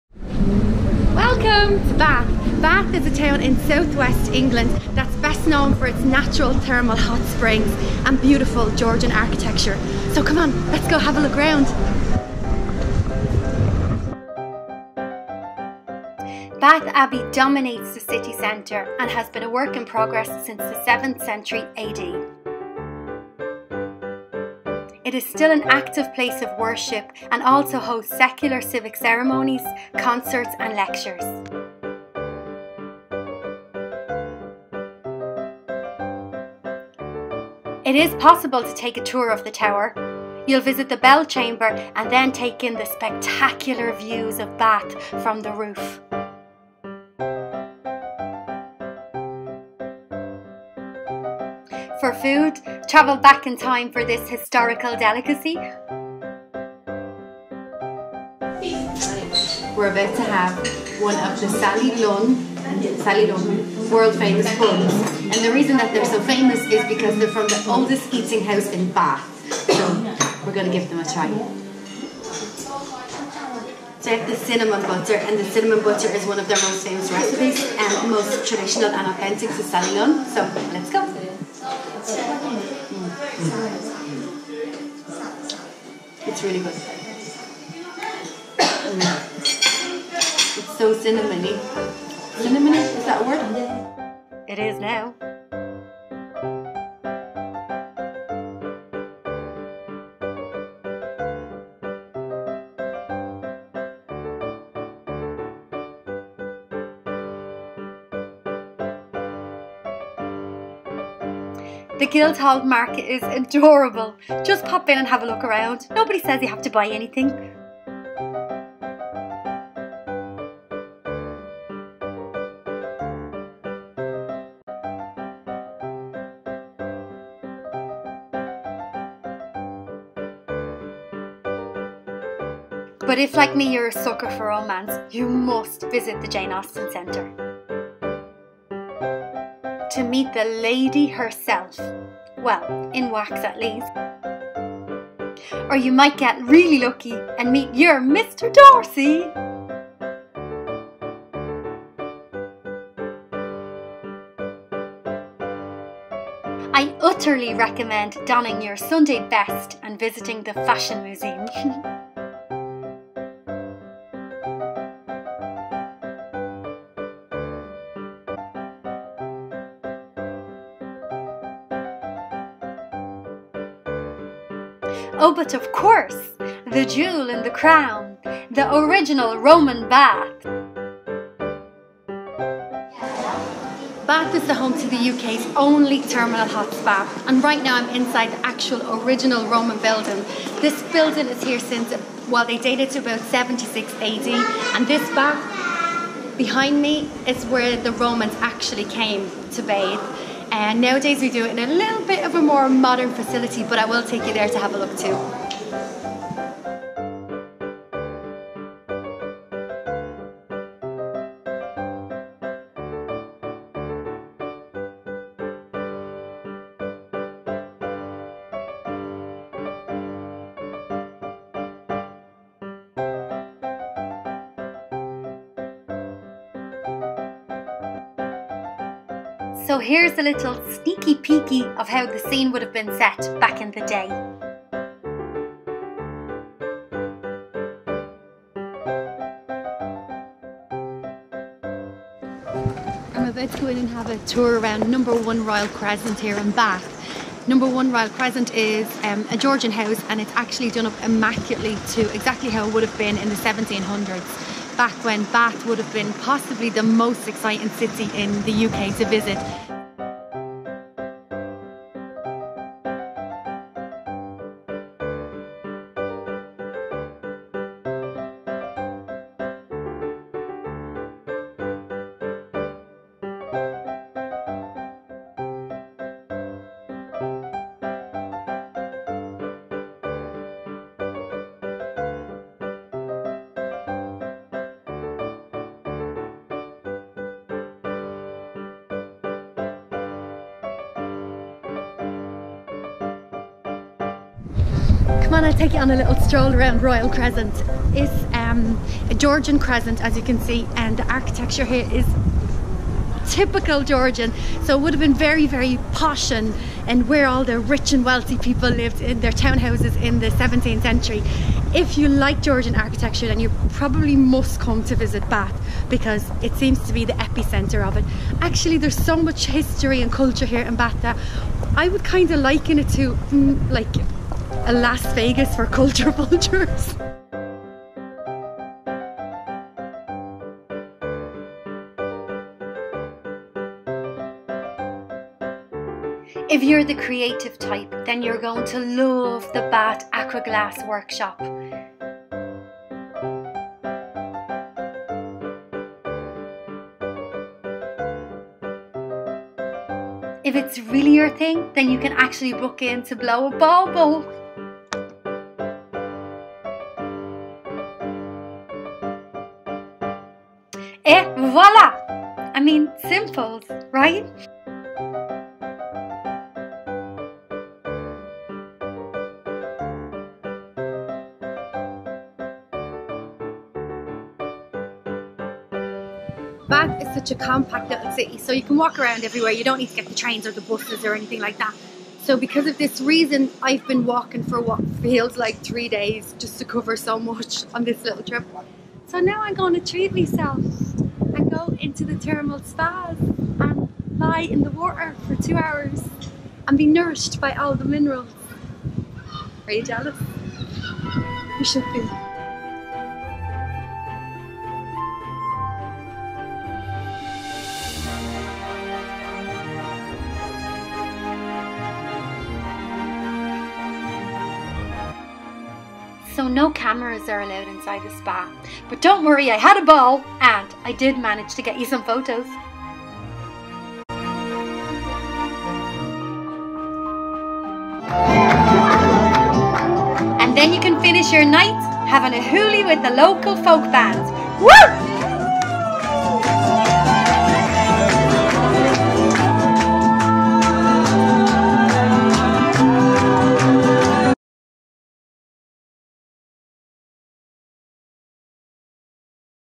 Bath, England - A Perfect Weekend Itinerary
Here's some inspiration on how to spend a few days in Bath, England.  I packed so many cultural, historical and tourist attractions into three days in Bath, UK.

If you're a Jane Austen fan then you'll love the Jane Austen Centre.  The views from Bath Abbey over the city are endless and the bunn from Sally Lunn's eating house are so very more'ish!

But don't stop there, with so many museums, art galleries and of course the ancient Roman Baths you'll be on your toes from morning til nights.  Then wind down and relax at the Thermae Spa in the mineral-rich waters to rejuvenate your body.